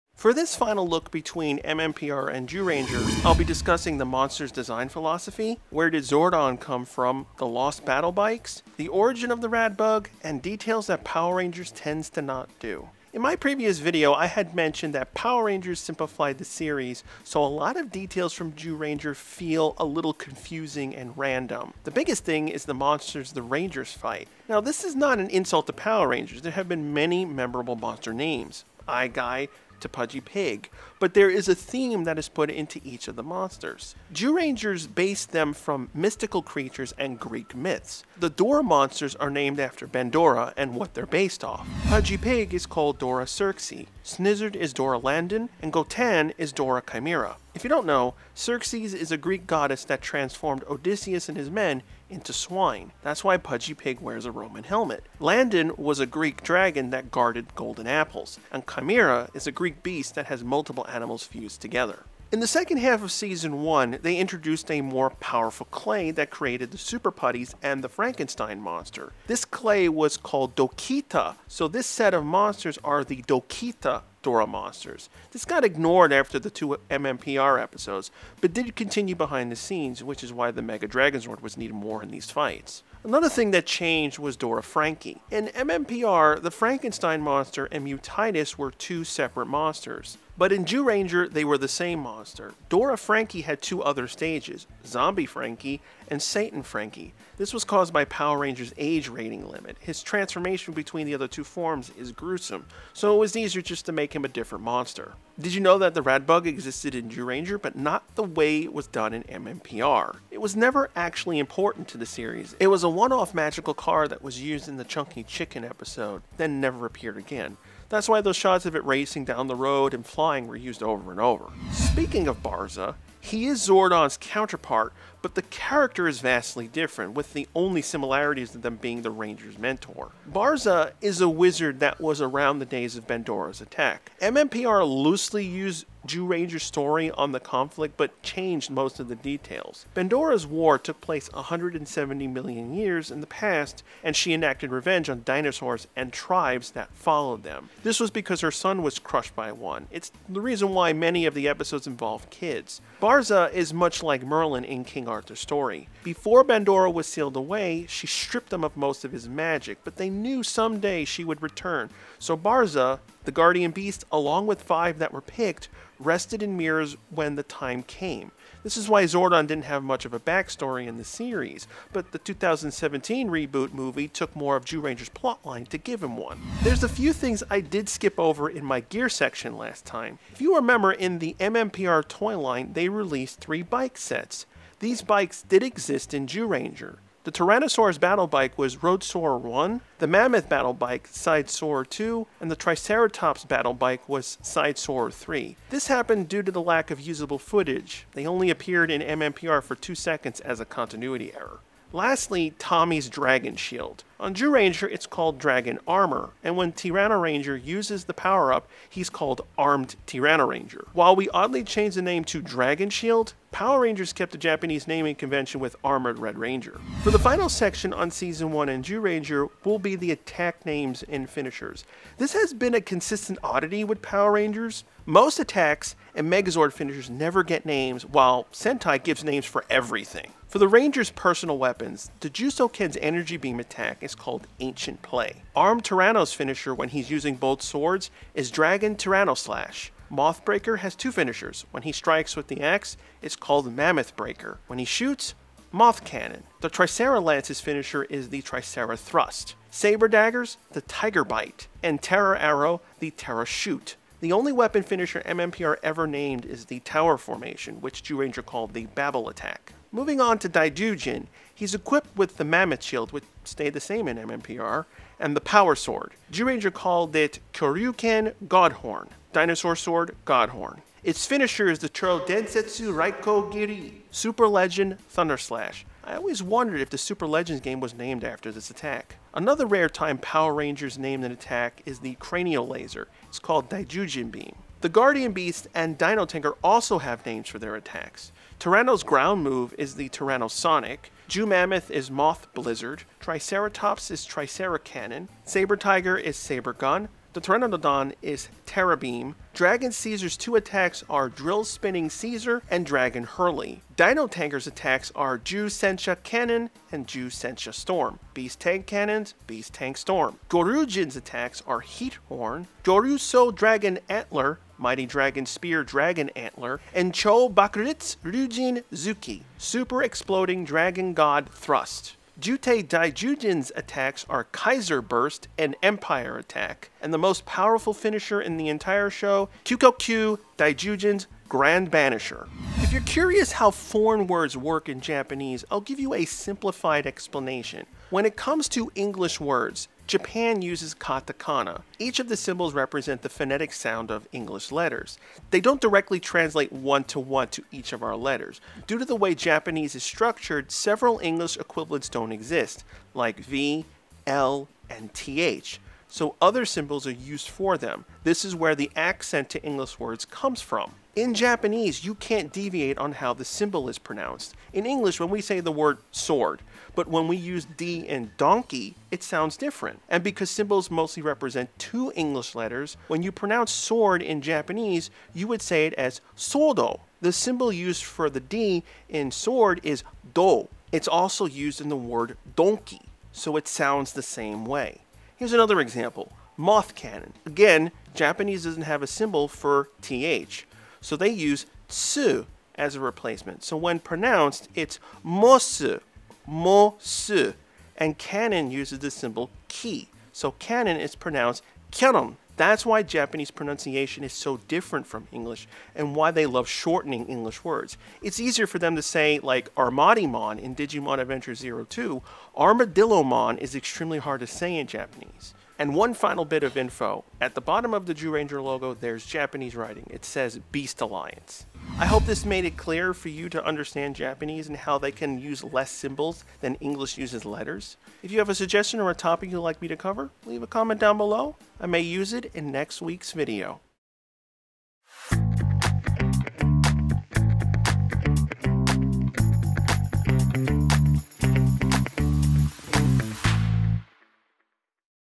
How Do They Make Power Rangers 101 For Beginner's - Battle Bikes, Barza, & Katakana (Episode 03)
In this final video on Zyuranger. I talk about Barza and the original story. The lost Battle Bikes. Where the RADBUG came from. Attacks and Finishers. And do a lesson on Katakana. Next Up, Dairanger.

- Filmed On: Panasonic FZ-300
- Edited On: Adobe Premiere Pro CS 2022
- Effects On: Adobe After Effects CS 2022